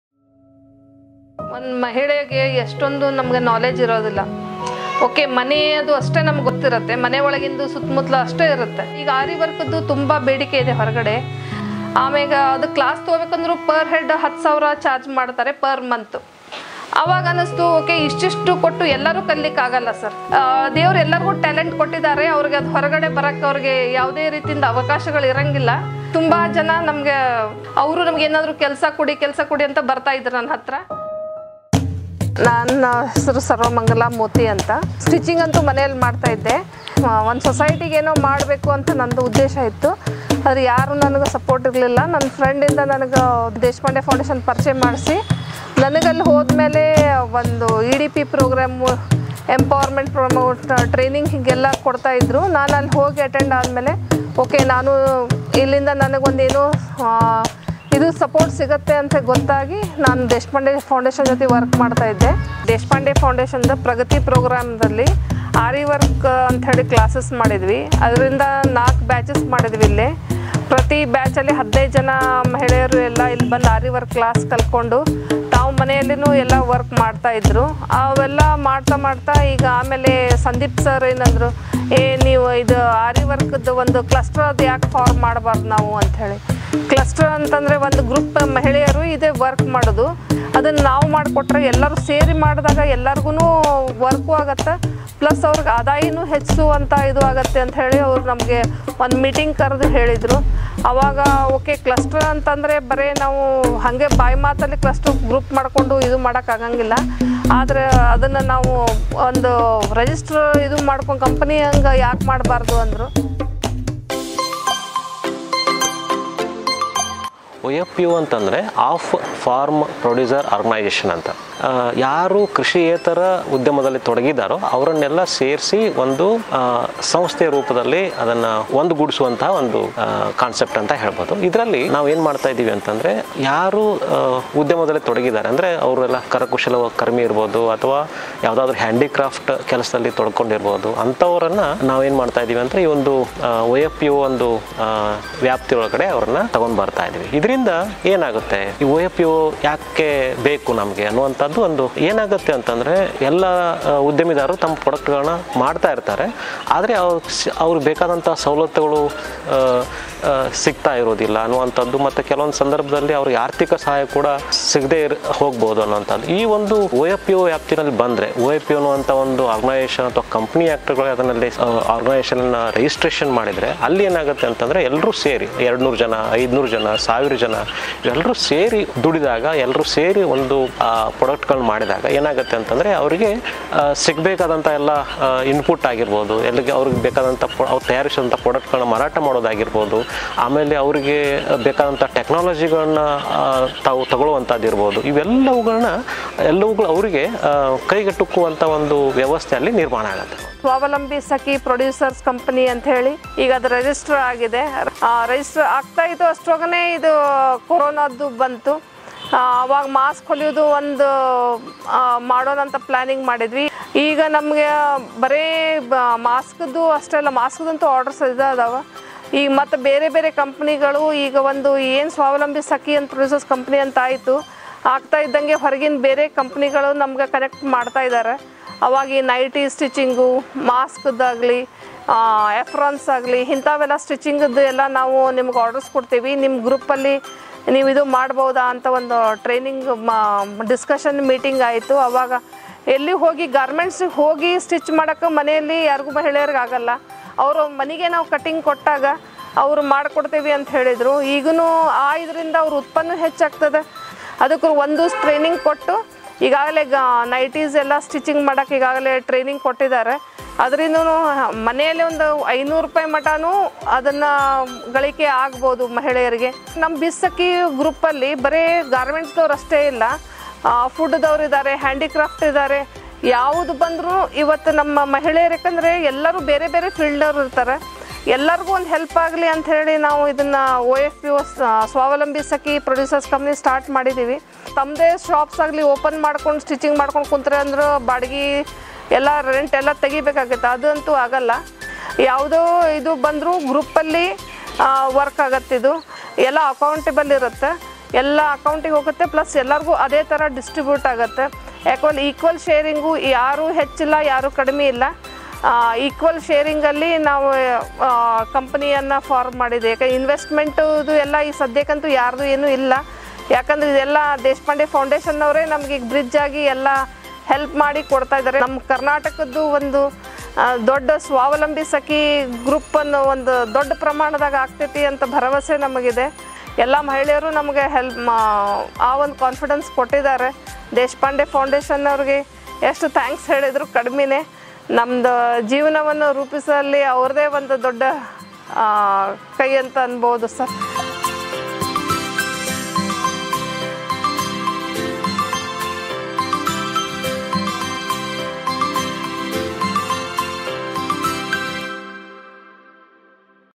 Deshpande Foundation | MEDP | OFPO
AARI CLUSTER
India has more than 25 million people working in the handlooms and handicrafts sector, almost a similar number of people in grassroots entrepreneurship making livelihoods ends meet out of entrepreneurial efforts.

These grassroots entrepreneurs are inhabited in clusters of villages or at the same geo-spatial habitats in semi-urban and urban areas. They can gain a better livelihood if some common needs may be attended to professionally:

There are pilots in Kolhapuri chappal making leather artisans, food processing and snacks making women producers, and AARI Embroidery women in North Karnataka taken up to help them evolve mutually aided, self-reliant Off-Farm Producers Organisations.